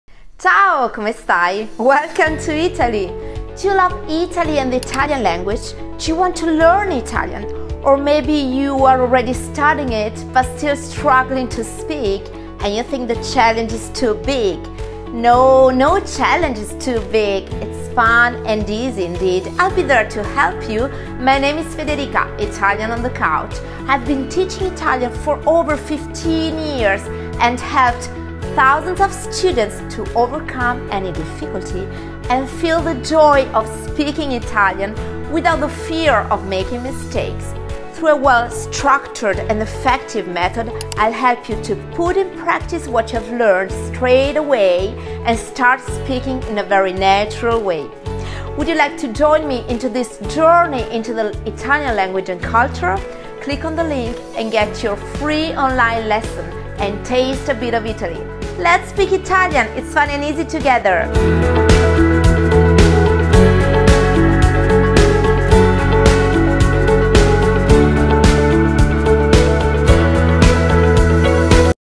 Let's speak Italian! New Groups Courses and a gift!
Are you in love with Italy and the Italian language?

Do you want to learn Italian?

Do you struggle speaking Italian and think the challenge is too big?

 No challenge is too big!

 Learning Italian together is fun and easy indeed.

Enroll to my Italian laguage courses and start speaking from the first lesson.

New Group Live Courses are starting soon

But spots are limited!

Reserve your spot now and the first lesson is free for you!

Send an email to
 and reserve your free Italian lesson.